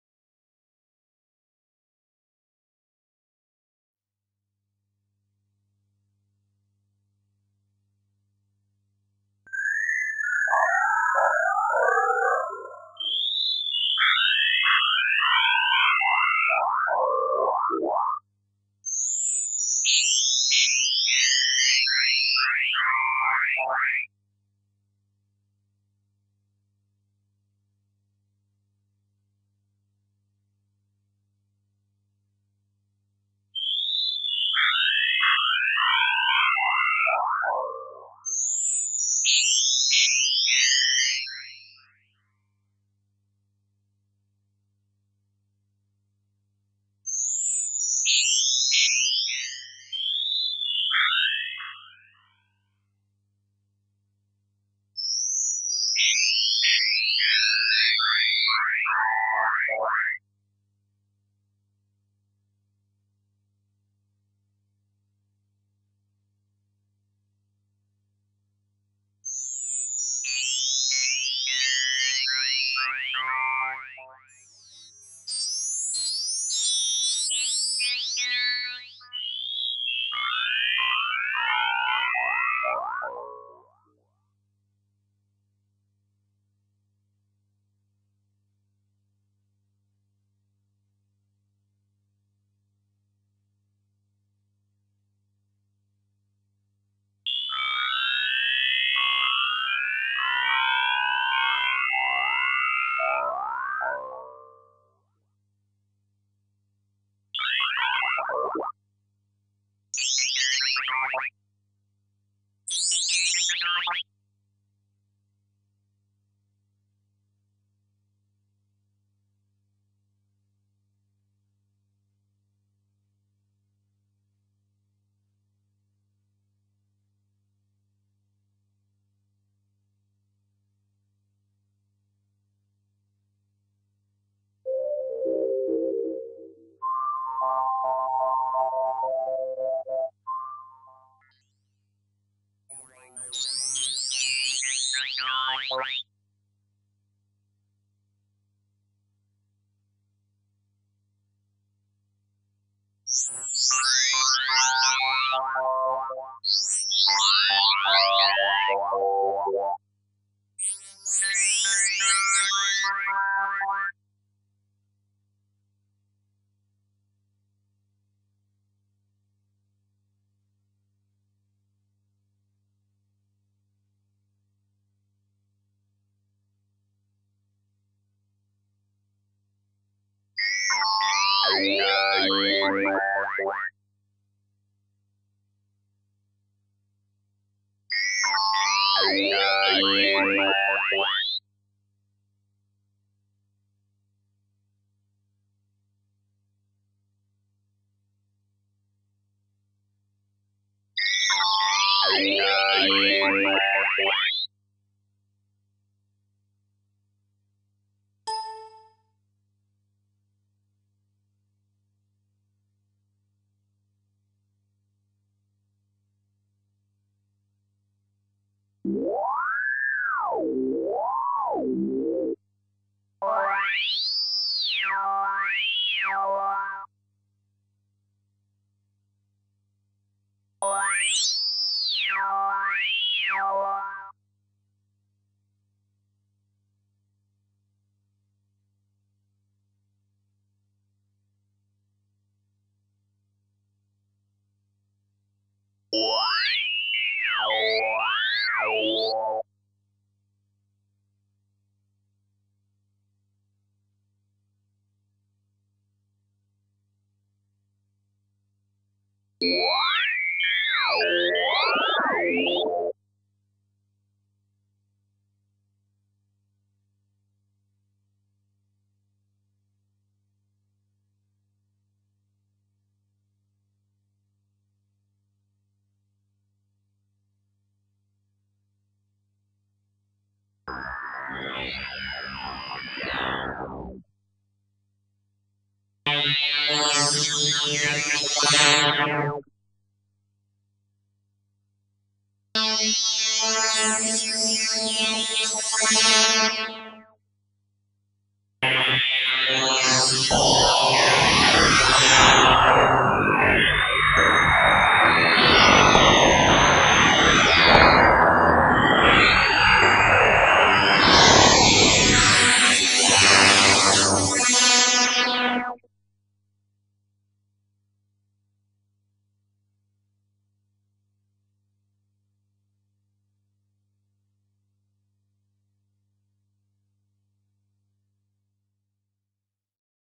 SpectroBits Demo
SpectroBits is a "Spectrogram" based Synhtesizer.
That means a Spectrum by time graph.
Generate sounds from any pictures.
Written as an entry into the KVR Developer Challenge 2009.